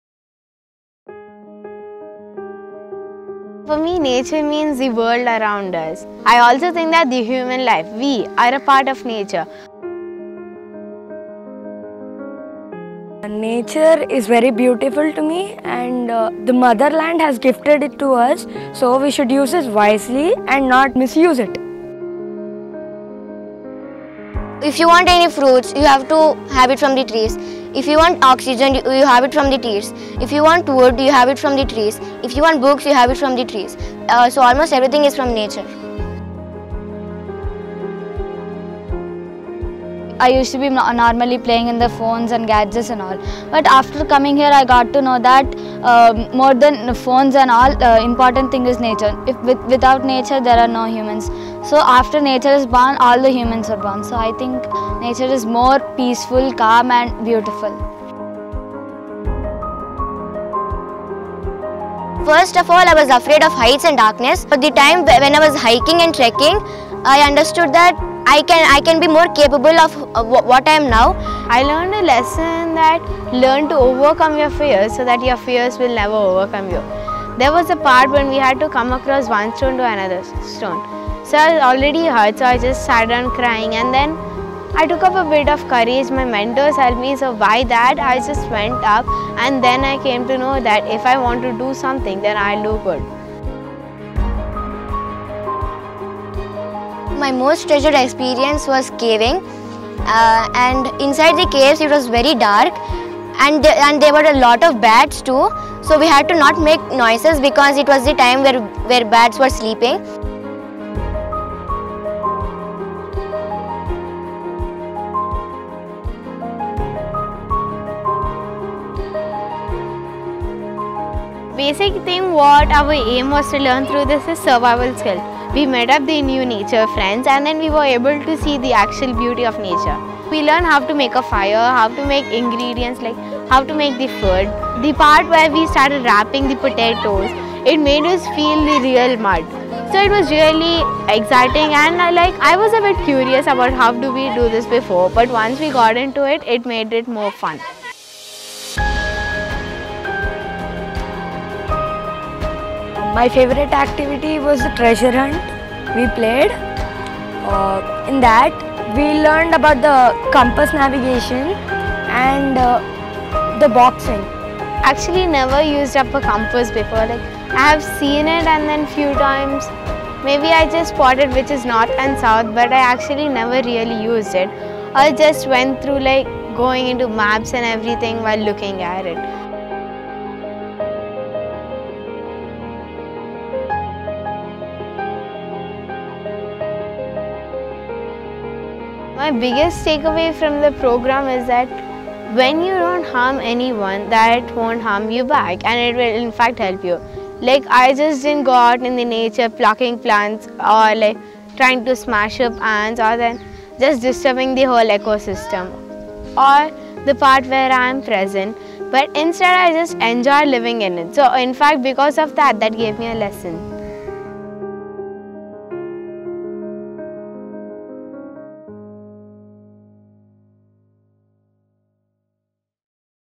Nature Turks - Lifelong Appreciation for Nature
In an era where we are largely disconnected from nature, it is important to make a real investment in understanding our environment. NatureTurks is a commitment to provide environmental education with an urban context to grow citizens who champion nature conservation. Hear it from young students what nature means to them.

Worldview's Nature Turks and Regenerative Education programs are delivered across campuses in India, for students of all ages. to learn more.

The video is an original production of Worldview Education Services Pvt Ltd.